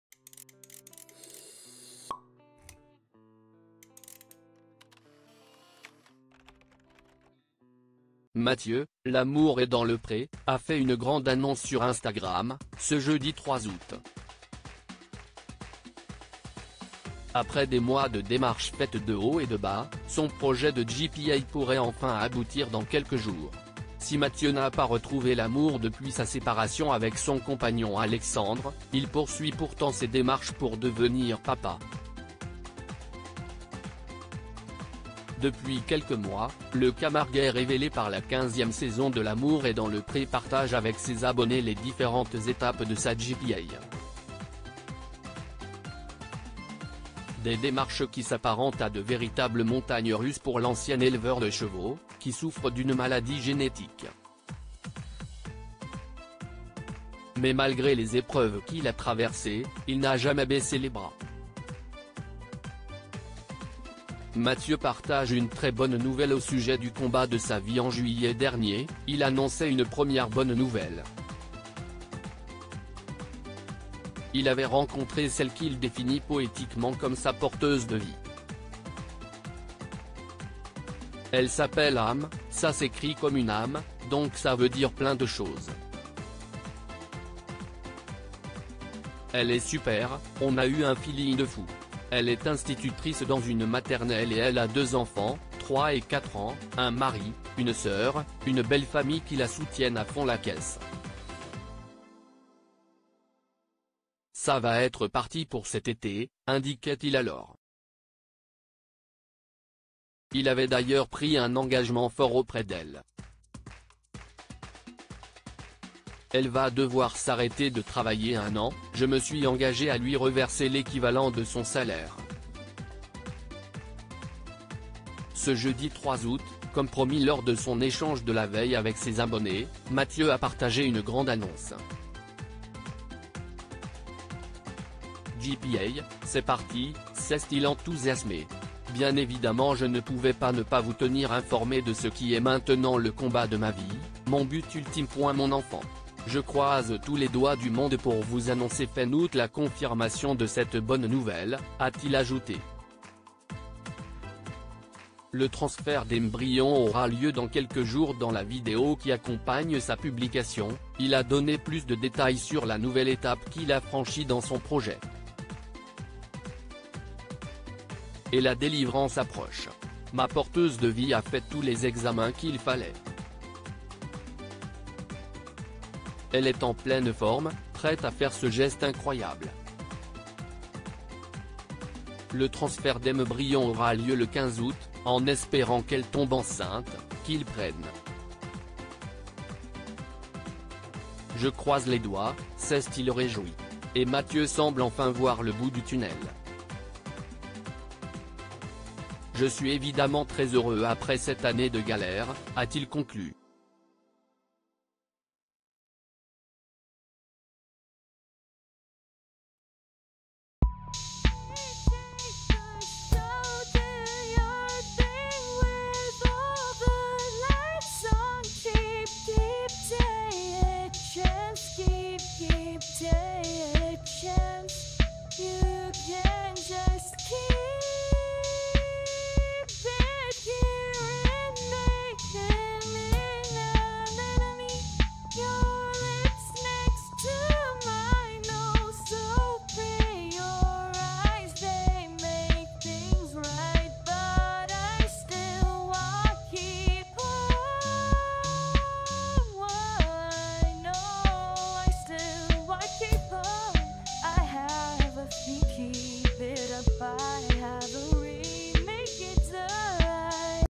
"C'est parti" : Mathieu (L'amour est dans le pré) annonce une bonne nouvelle au sujet de son projet
"C'est parti" : Mathieu (L'amour est dans le pré) annonce une bonne nouvelle au sujet de son projet de GPA qui pourrait enfin aboutir dans un avenir très proche